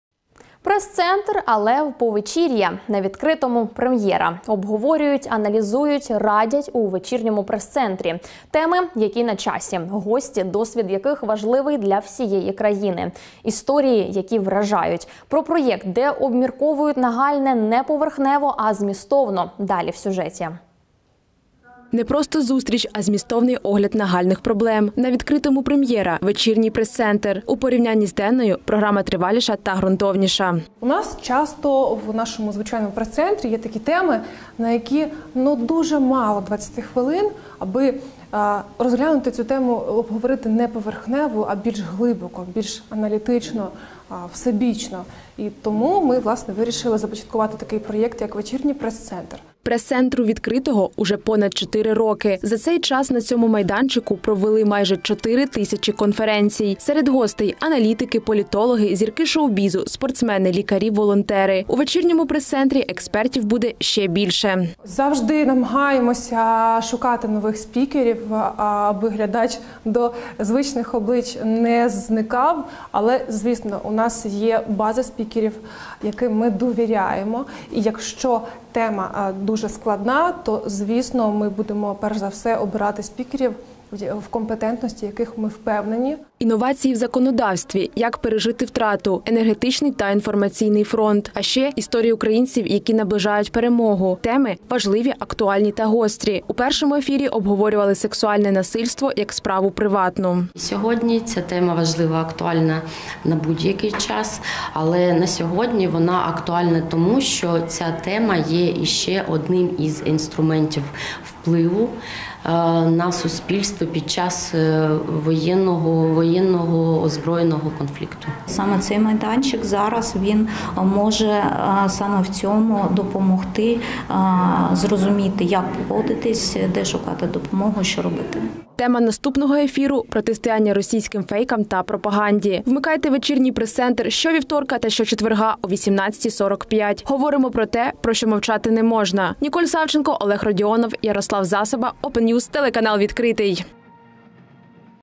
«Вечірній пресцентр»: на телеканалі «Відкритий» стартував новий проєкт
Пресцентр – але в повечір'я. На Відкритому – прем’єра. Обговорюють, аналізують, радять – у Вечірньому пресцентрі. Теми – які на часі. Гості – досвід, яких важливий для всієї країни. Історії – які вражають. Про проєкт, де обмірковують нагальне не поверхнево, а змістовно, – далі в сюжеті. _______________________________________________